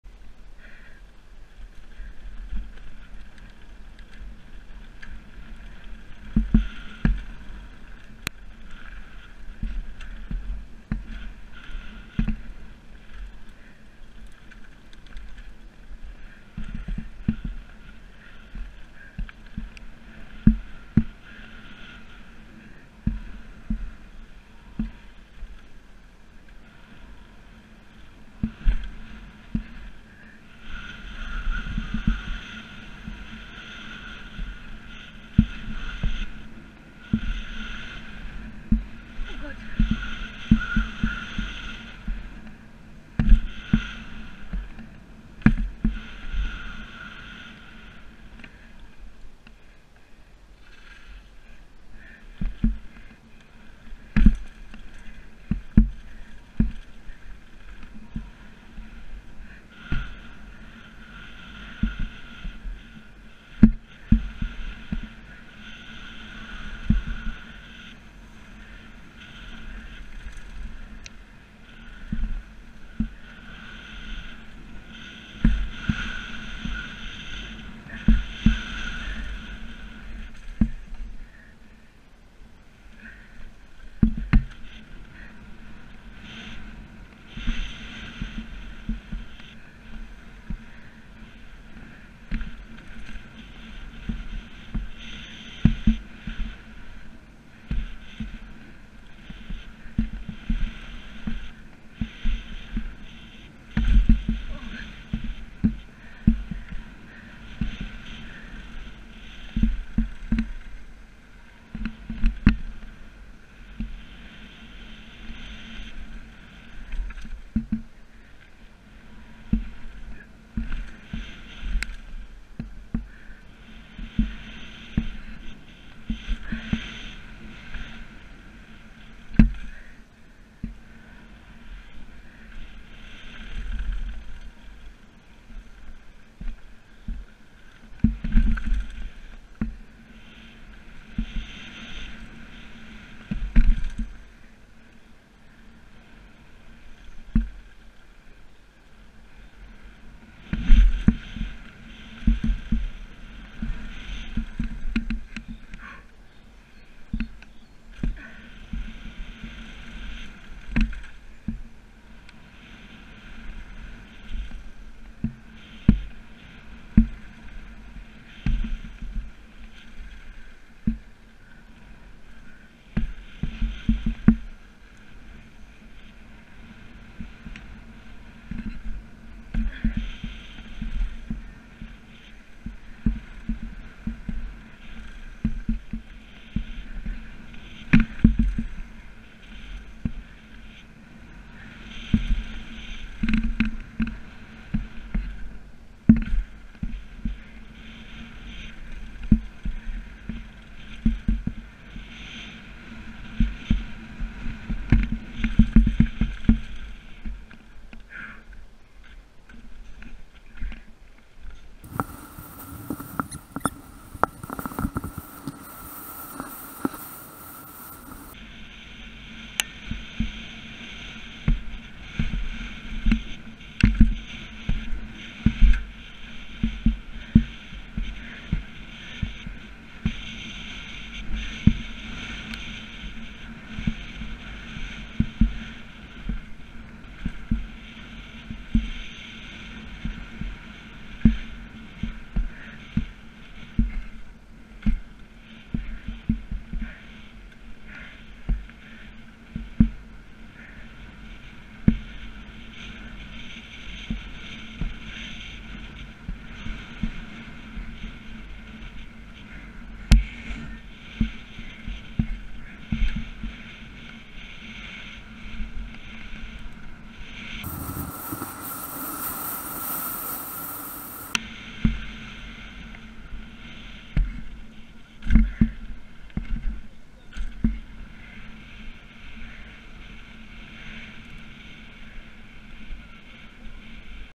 MTB-Zone Willingen FREERIDE | Full Run | Mai 2018 | RAW
Moin, das ist ein kleines Video (also eigentlich die ganze Strecke) von der Freeride-Strecke in Willingen. Ich hoffe euch gefällt`s.